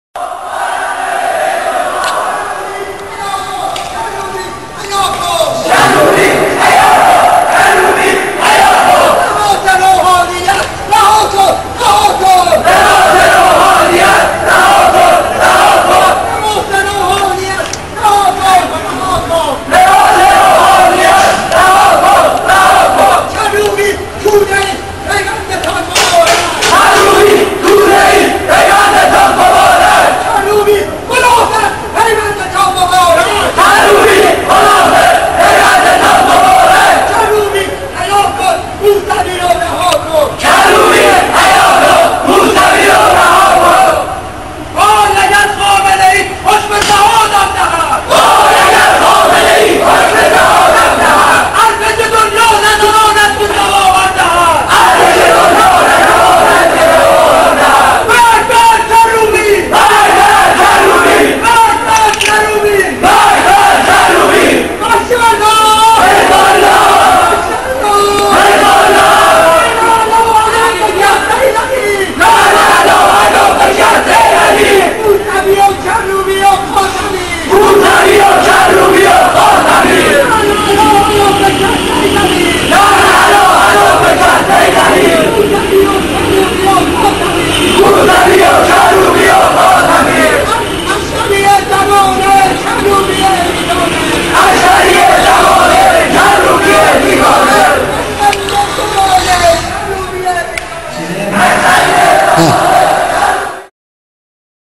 حمله به منزل مهدی کروبی - چهارشنبه ۱۰ شهریور
فیلم تازه منتشر شده از حمله به منزل مهدی کروبی - چهارشنبه ۱۰ شهریور